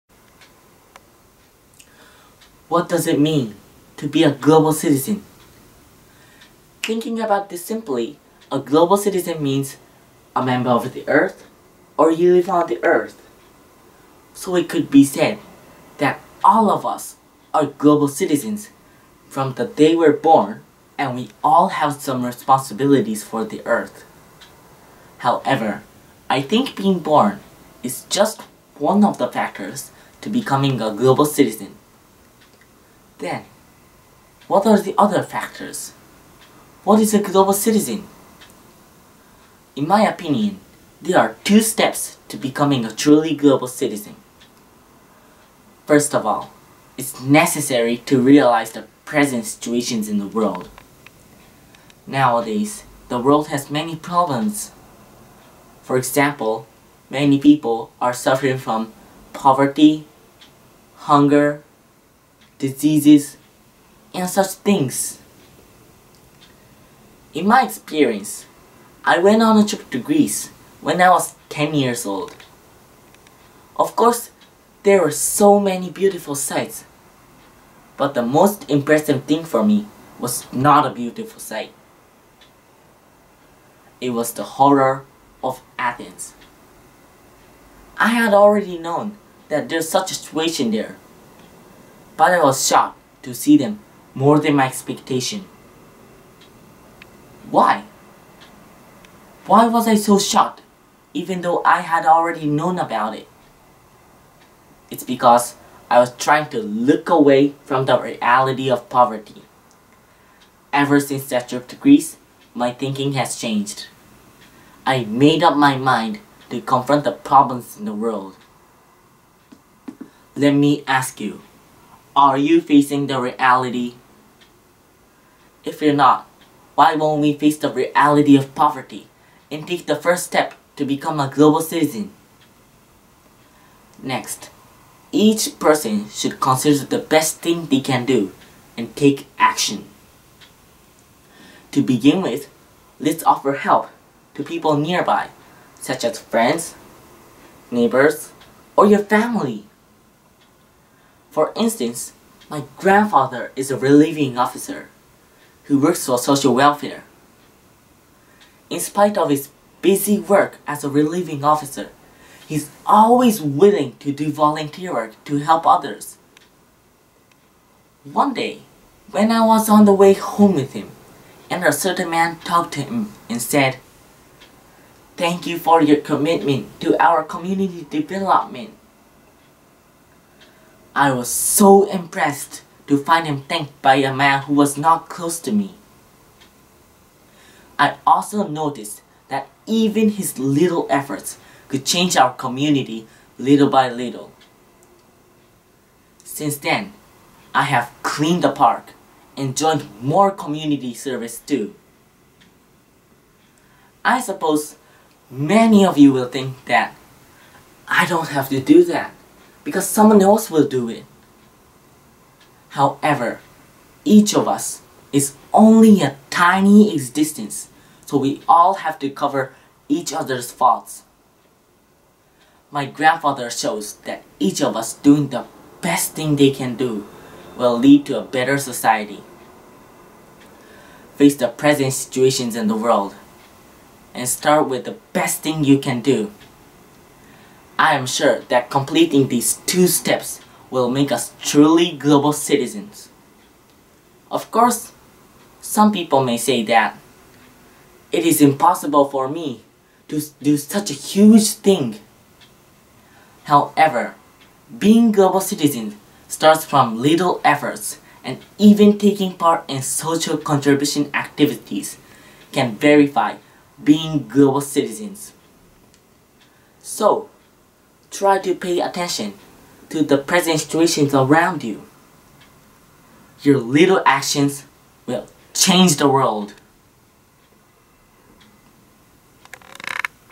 EF Challenge 2017 -Toshiaki Tachibana - Japan -
Public Speaking at Koyo Gakuin Junior High School

Featuring all the winners.

Featuring winners from Japan.